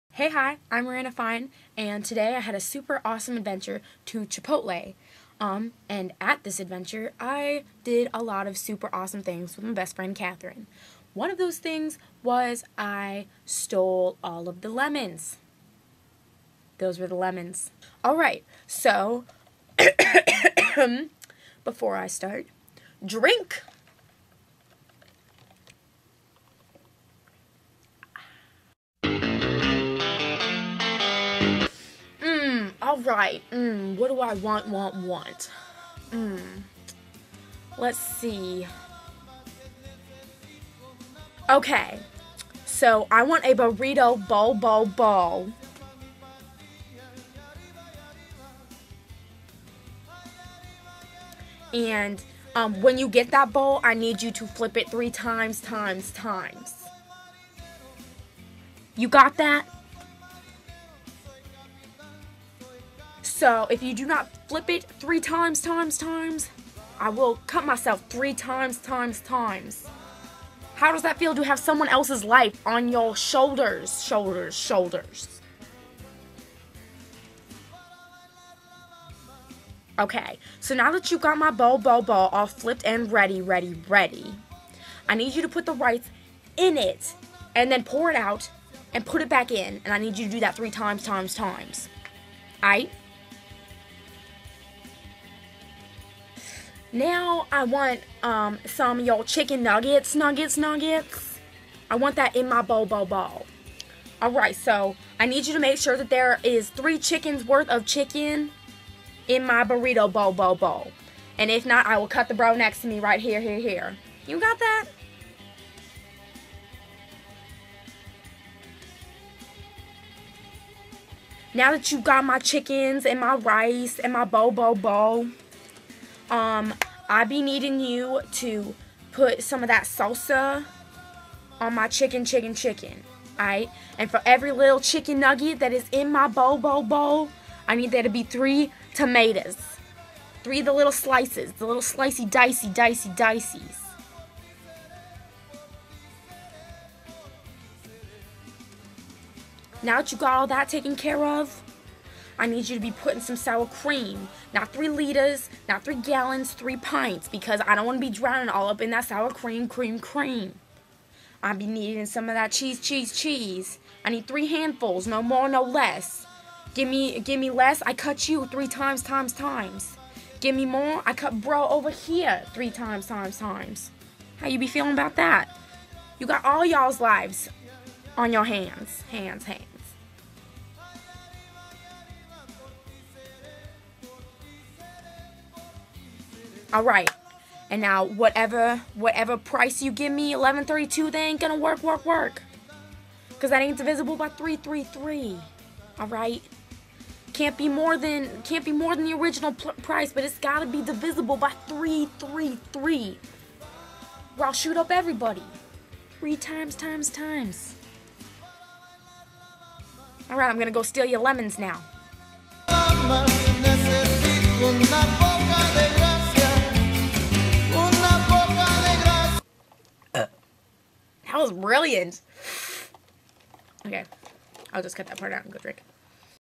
Chipotle Chipotle Chipotle
This Is How I Order My Food From Chipotle, Thanks To My Severe OCD.
This Video Also Features Tony, The No-Funny-Business Mexican.